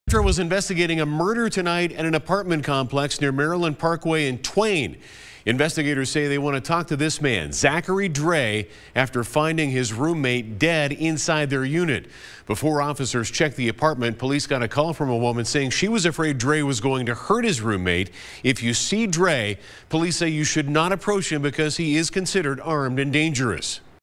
Las Vegas police looking for suspect in Maryland Parkway homicide investigation
Zachary Drey is not in custody but should be considered armed and dangerous.